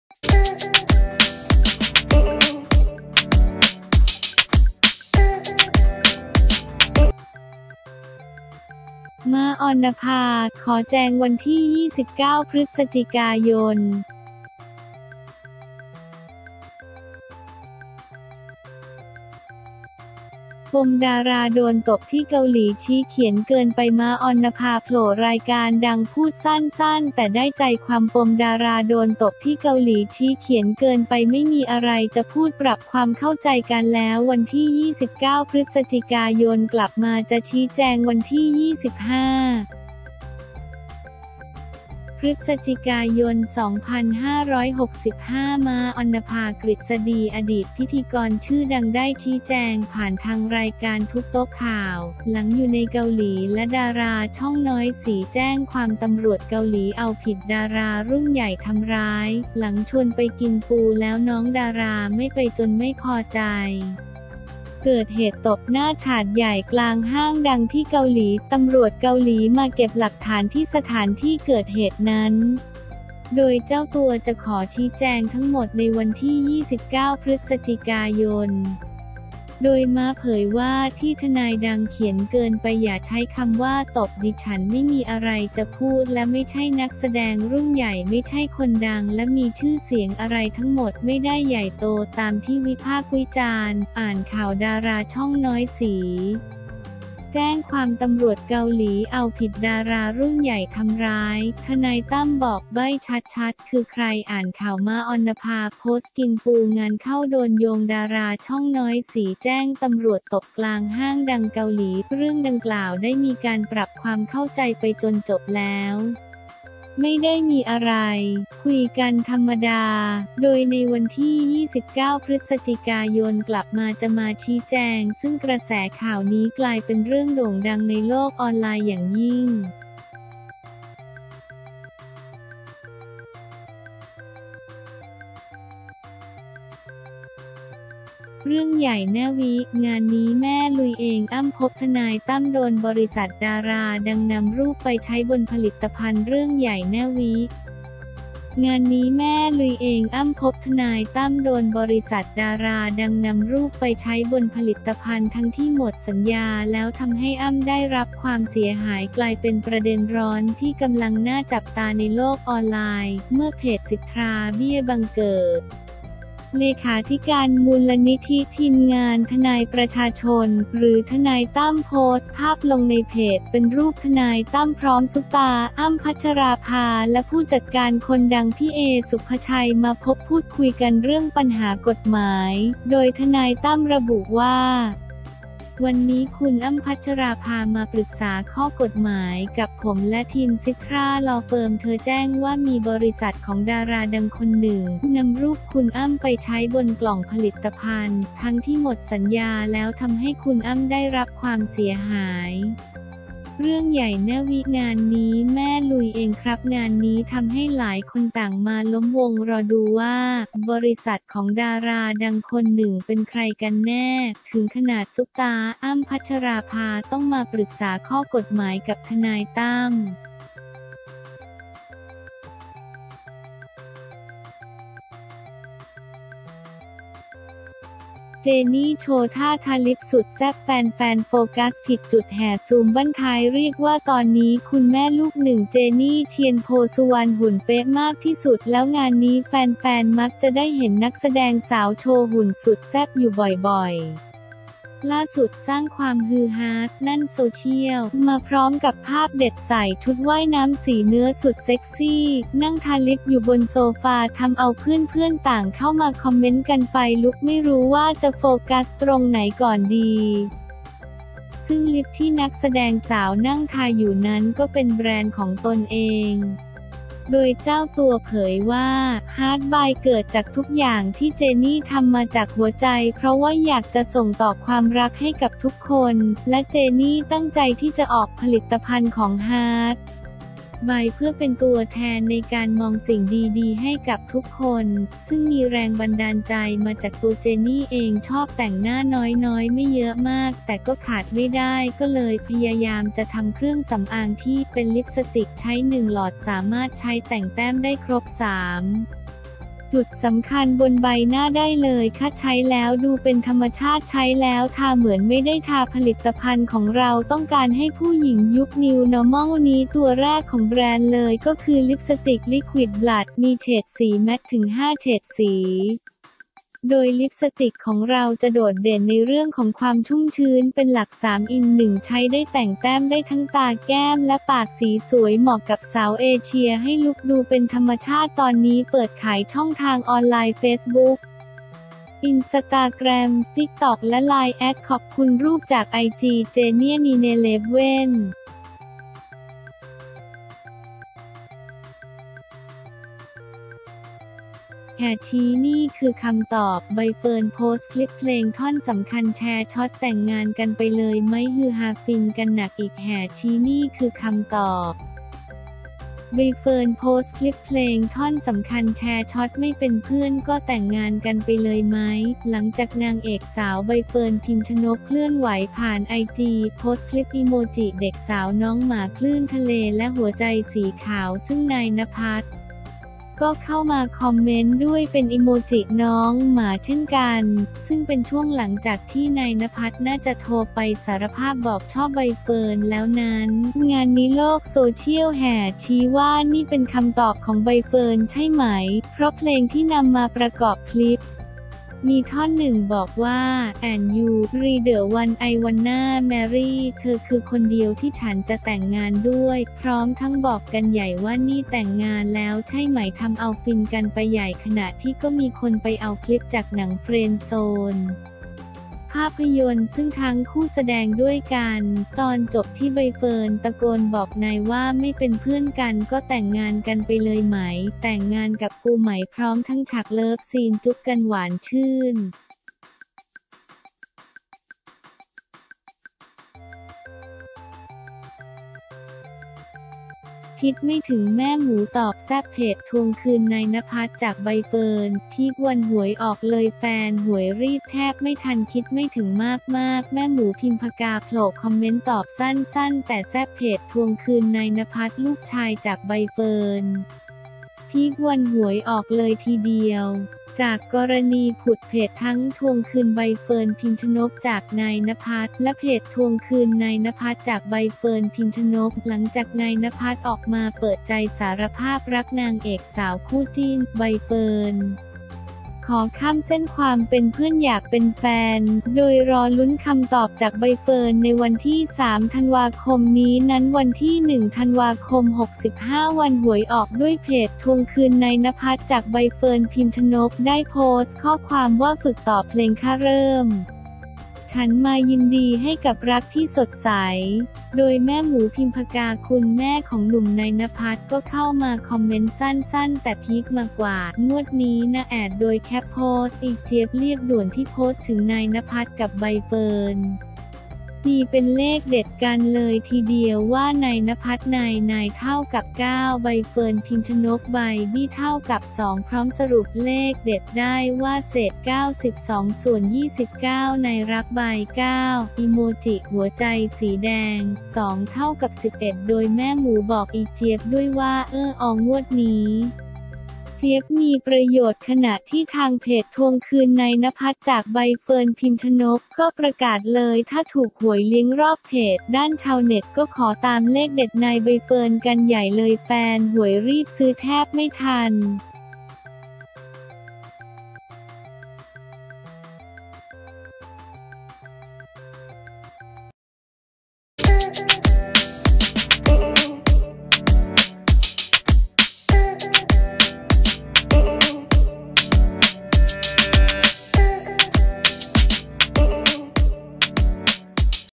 'ม้า อรนภา' ขอแจง วันที่ 29 พ ย ปมดาราโดนตบที่เกาหลี ชี้เขียนเกินไป
ม้า อรนภาโผล่รายการดัง พูดสั้นๆ แต่ได้ใจความ ปมดาราโดนตบที่เกาหลี ชี้เขียนเกินไป ไม่มีอะไรจะพูด ปรับความเข้าใจกันแล้ว วันที่ 29 พ.ย.กลับมาจะชี้แจง
วันที่ 25 พ.ย.2565 ม้า อรนภา กฤษฎี อดีตพิธีกรชื่อดัง ได้ชี้แจงผ่านทางรายการทุบโต๊ะข่าว หลังอยู่ในเกาหลี และดาราช่องน้อยสี แจ้งความตร.เกาหลีเอาผิดดารารุ่นใหญ่ทำร้าย หลังชวนไปกินปูแล้วน้องดาราไม่ไปจนไม่พอใจ เกิดเหตุตบหน้าฉาดใหญ่กลางห้างดังที่เกาหลี ตำรวจเกาหลีมาเก็บหลักฐานที่สถานที่เกิดเหตุนั้น โดยเจ้าตัวจะขอชี้แจงทั้งหมดในวันที่ 29 พ.ย.
โดยม้าเผยว่า ที่ทนายดังเขียนเกินไป อย่าใช้คำว่าตบ ดิฉันไม่มีอะไรจะพูด และไม่ใช่นักแสดงรุ่นใหญ่ ไม่ใช่คนดัง และมีชื่อเสียงอะไร ทั้งหมดไม่ได้ใหญ่โตตามที่วิพากษ์วิจารณ์
อ่านข่าว ดาราช่องน้อยสี แจ้งความตร.เกาหลีเอาผิดดารารุ่นใหญ่ทำร้าย ทนายตั้ม บอกใบ้ชัดๆ คือใคร
อ่านข่าว ม้า อรนภา โพสต์กินปู งานเข้าโดนโยงดาราช่องน้อยสีแจ้งตร.ตบกลางห้างดังเกาหลี

เรื่องดังกล่าวได้มีการปรับความเข้าใจไปจนจบแล้ว ไม่ได้มีอะไร คุยกันธรรมดา โดยในวันที่ 29 พ.ย.กลับมาจะมาชี้แจง ซึ่งกระแสข่าวนี้กลายเป็นเรื่องโด่งดังในโลกออนไลน์อย่างยิ่ง